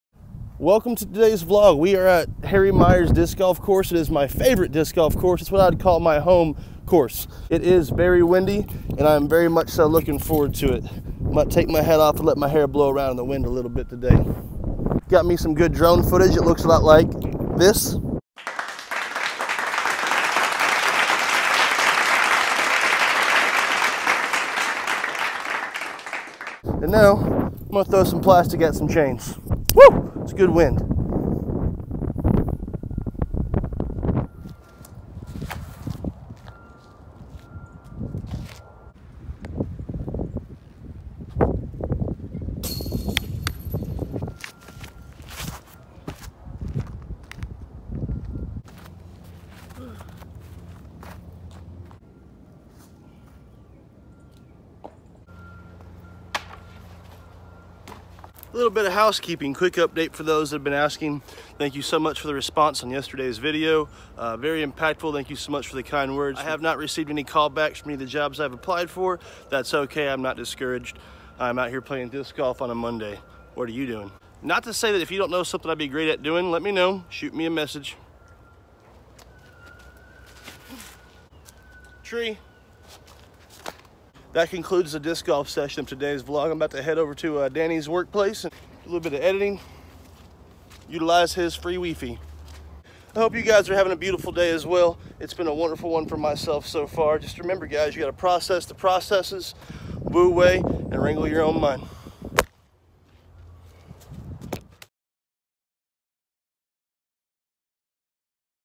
Harry Myers DGC
Daily Vlog 5. Windy day in Rockwall TX. Certainly have some rust to knock off.